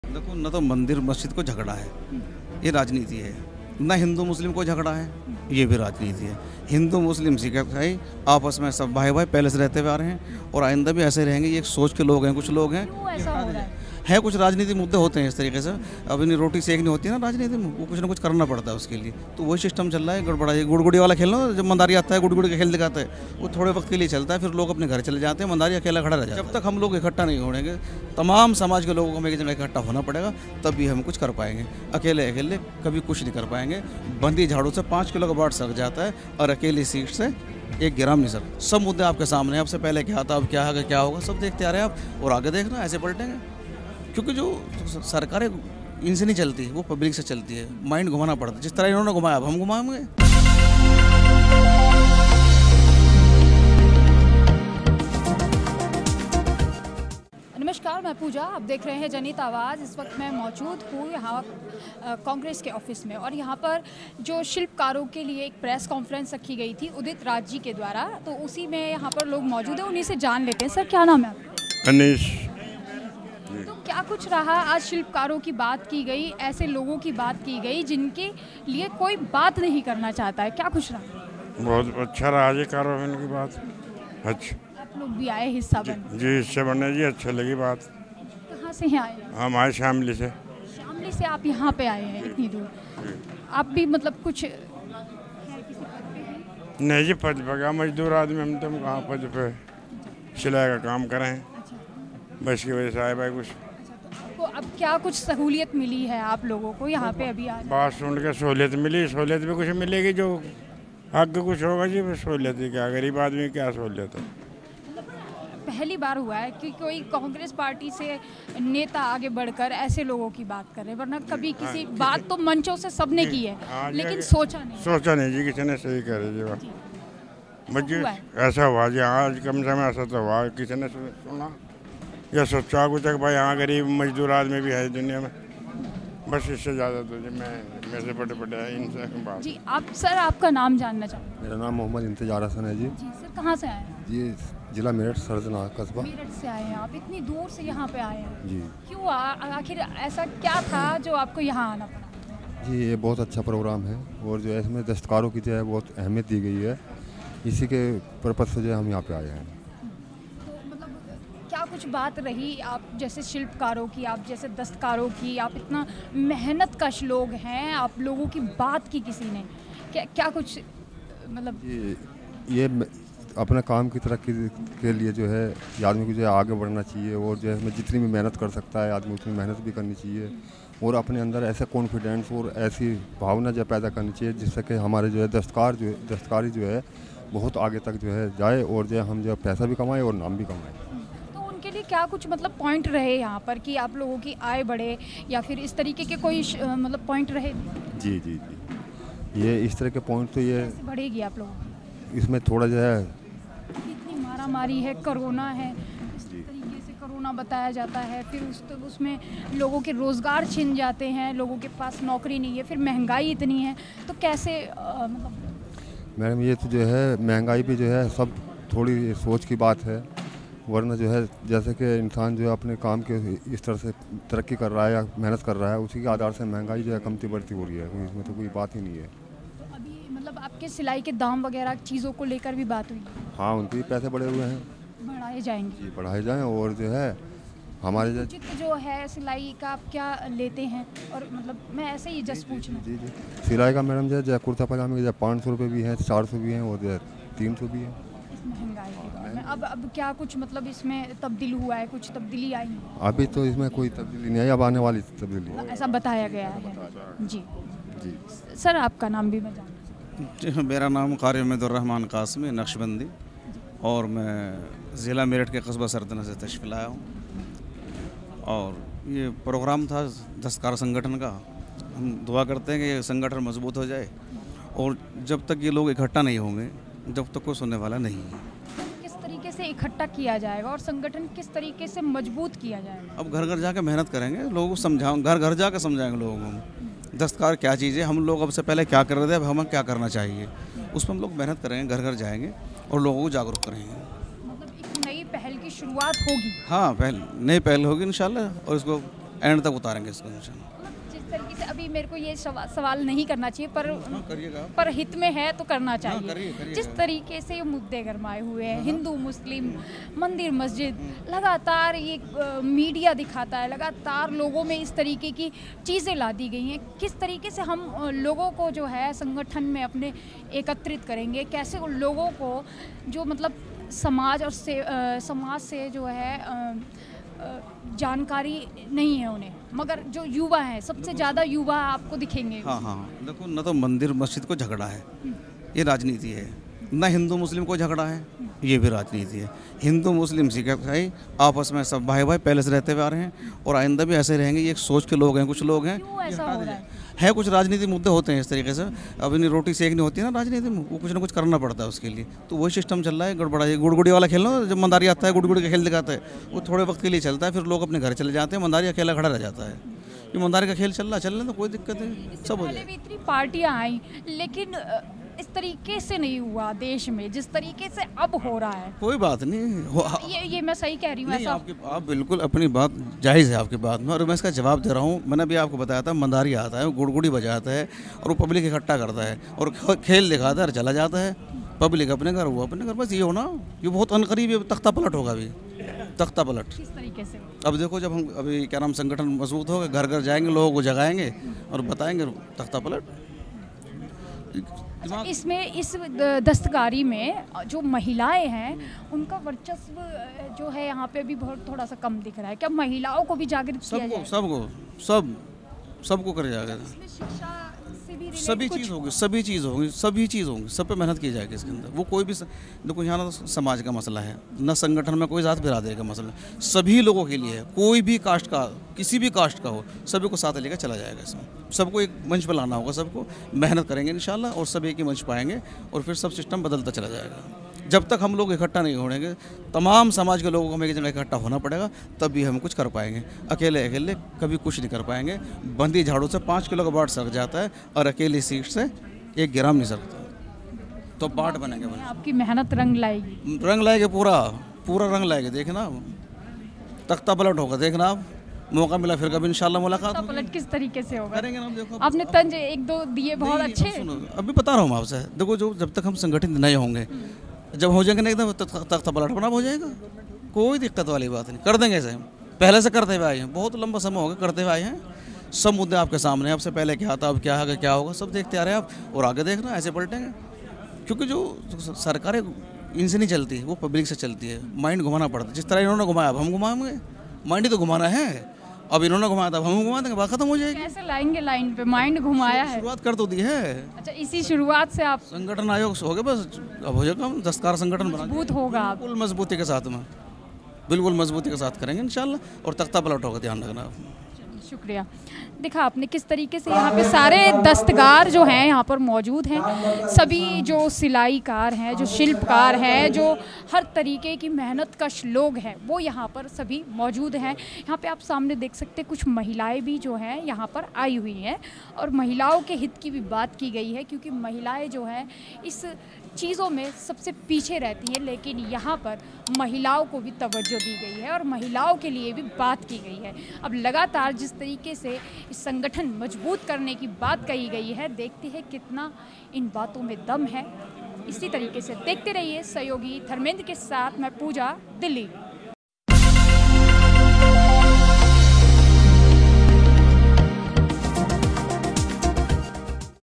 मौलाना ने तोड़ी चुप्पी कहा अब मदारी के खेल का होगा तख़्ता पलट / Chairman Parisangh DR Udit Raj
मौलाना ने तोड़ी चुप्पी कहा अब मदारी के खेल का होगा तख़्ता पलट / National Chairman Parisangh | DR Udit Raj
all _india_parisangh_#national _chairmen_parisangh_all _india